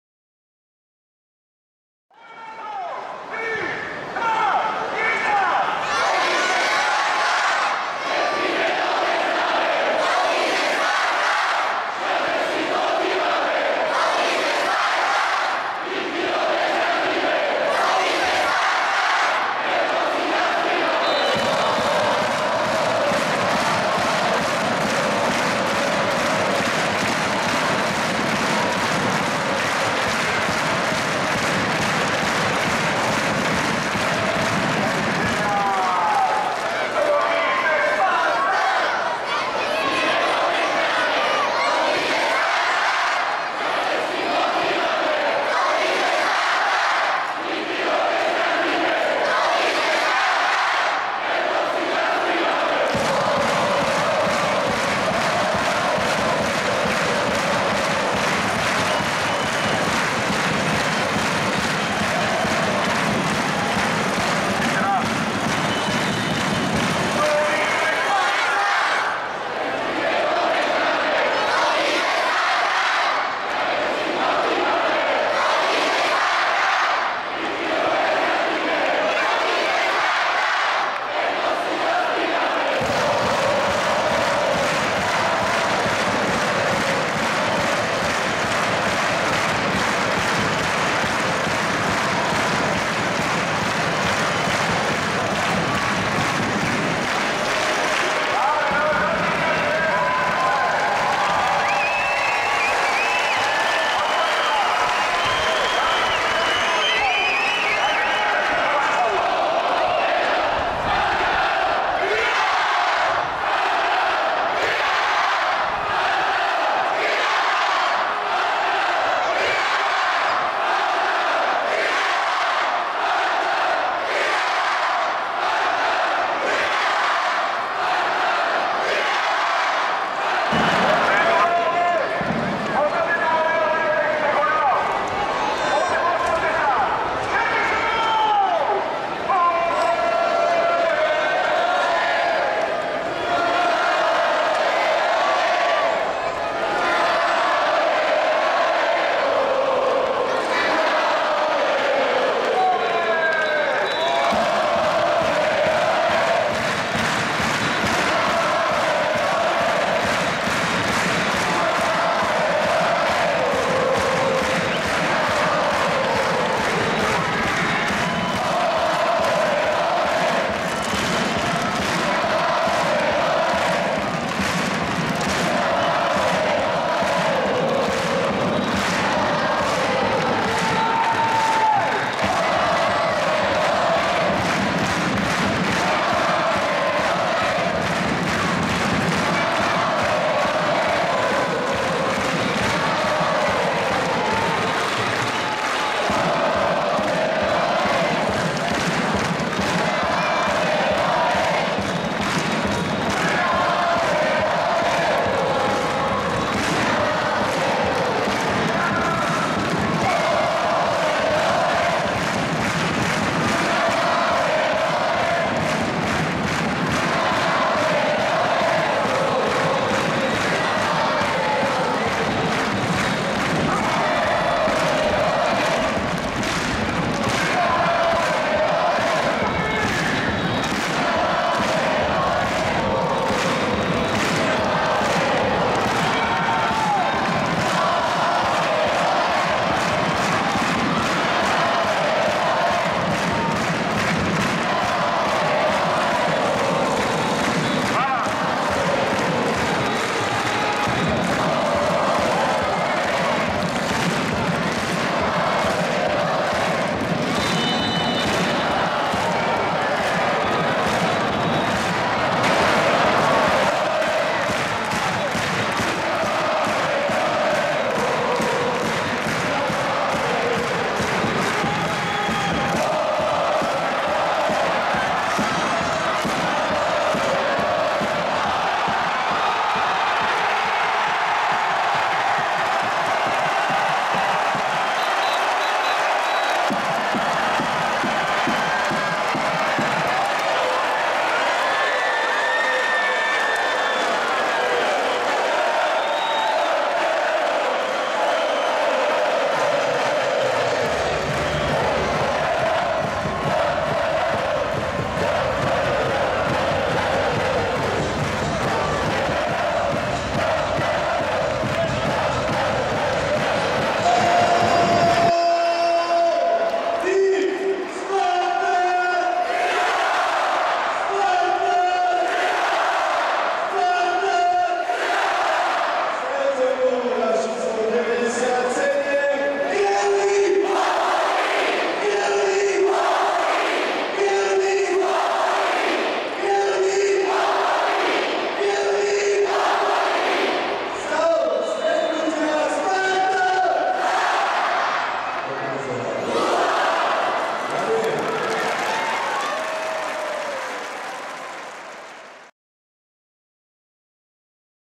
20240518 FC Spartak Trnava Detsky sektor
V poslednom kole ligy bojoval Spartak o účasť v Európskej konferenčnej lige. Veľkou mierou k výhre 5:0 proti Podbrezovej prispeli aj diváci v detskom sektore, deti a ich rodičia. Aj im patrí veľké poďakovanie, ktoré vyslovil na pozápasovej tlačovke tréner Spartaka Michal Gašparík.